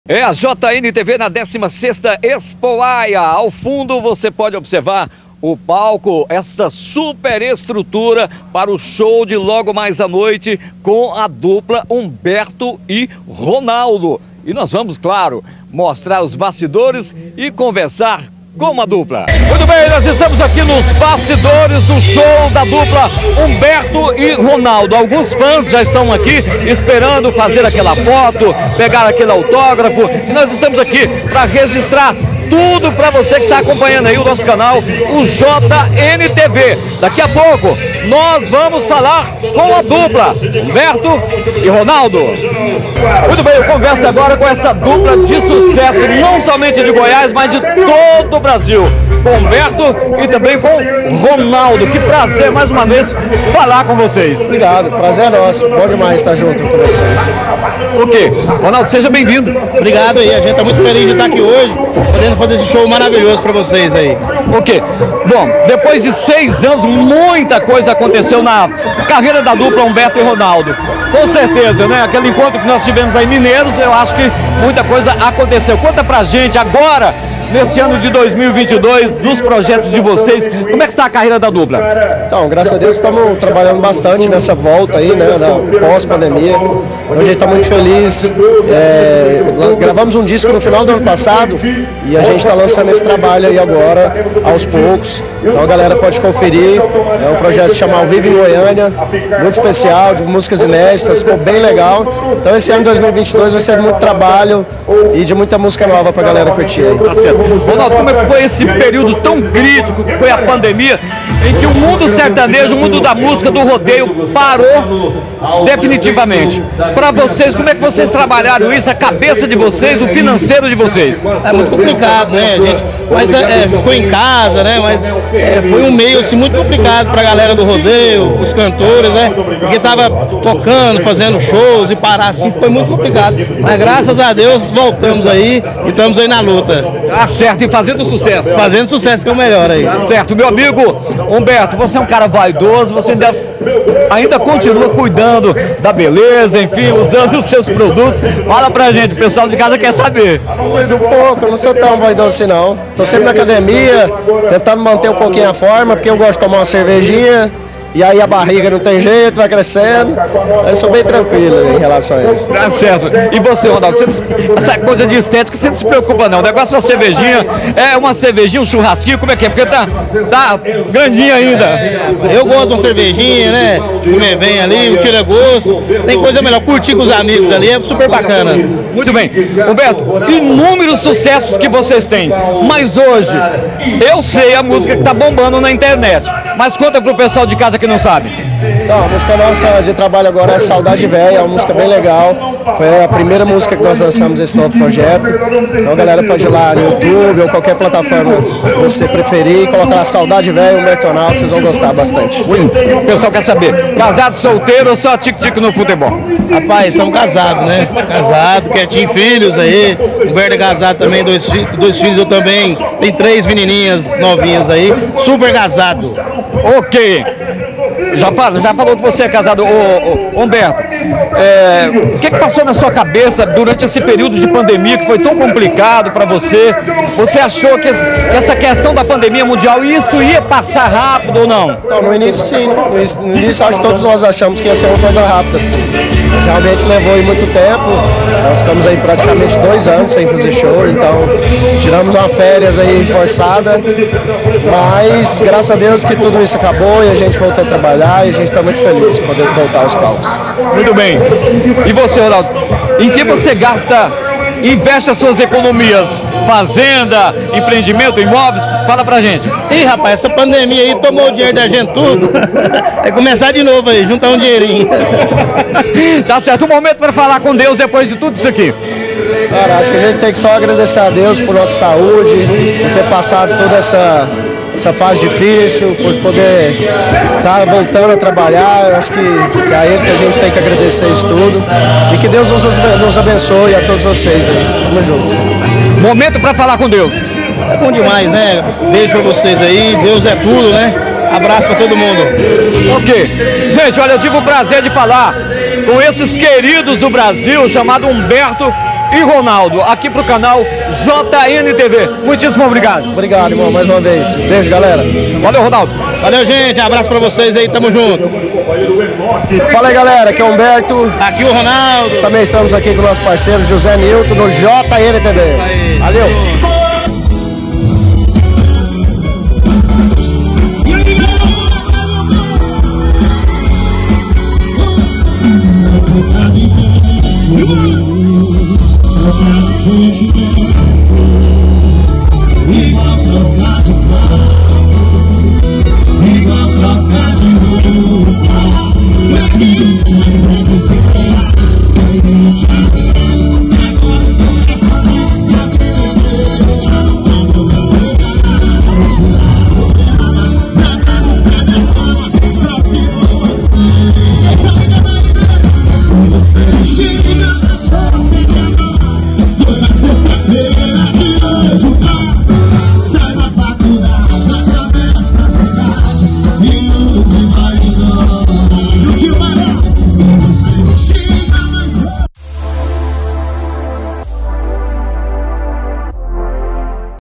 O QUE ACONTECEU COM A DUPLA HUMBERTO E RONALDO APÓS PANDEMIA. OS GOIANOS CONTINUAM FAZENDO SUCESSO?
Neste vídeo o jornalista José Nildo falou com a dupla Humberto e Ronaldo durante a festa da Expoaia. José Nildo neste vídeo mostrou os bastidores do show de Humberto e Ronaldo. Veja como foi  o show de dos goianos Humberto e  Ronaldo na 16ª Expoaia 2022. Veja como está hoje a dupla Humberto e Ronaldo de Goiás.  José Nildo falou com a dupla de sucess Humberto e Ronaldo neste vídeo. Quer ficar sabendo como está Humberto e Ronaldo e só assistir este vídeo com José Nildo.